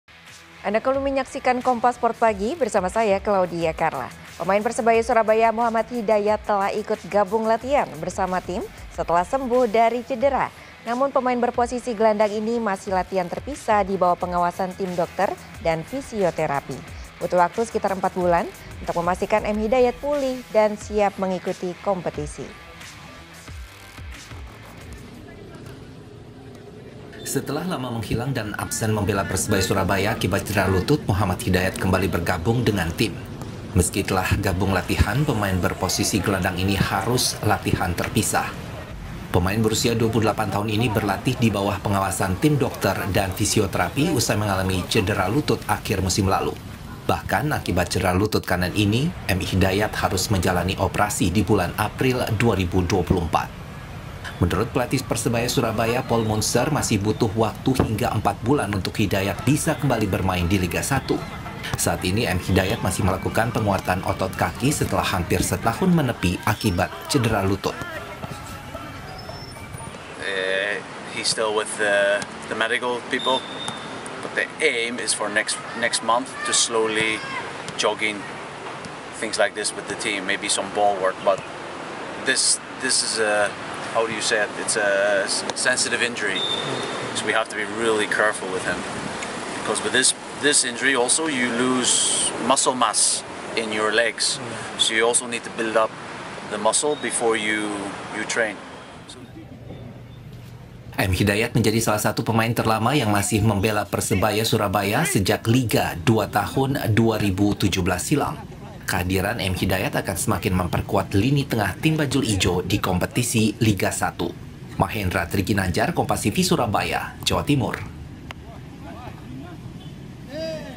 Sempat Absen Akibat Cedera Lutut, M Hidayat Kembali Gabung Latihan dengan Persebaya
SURABAYA, KOMPAS.TV - Setelah lama menghilang dan absen membela Persebaya Surabaya akibat cedera lutut, Muhammad Hidayat kini kembali bergabung dengan tim.

Meskipun telah bergabung dalam latihan, pemain berposisi gelandang ini masih menjalani latihan terpisah.

Pemain berusia 28 tahun ini berlatih di bawah pengawasan tim dokter dan fisioterapi setelah mengalami cedera lutut di akhir musim lalu.

Akibat cedera lutut kanan yang dideritanya, M. Hidayat harus menjalani operasi pada bulan April 2024.

Menurut pelatih Persebaya Surabaya, Paul Munster, Hidayat masih membutuhkan waktu hingga empat bulan untuk bisa kembali bermain di Liga Satu.

Saat ini, M. Hidayat sedang fokus pada penguatan otot kaki setelah hampir setahun absen akibat cedera lutut.

M. Hidayat merupakan salah satu pemain terlama yang masih membela Persebaya Surabaya sejak Liga Dua tahun 2017.

Kehadirannya diharapkan dapat semakin memperkuat lini tengah tim Bajul Ijo di kompetisi Liga Satu.

Baca Juga Link Live Streaming Persebaya Surabaya vs PSM Makassar, Kick-off Jam 19.00 WIB di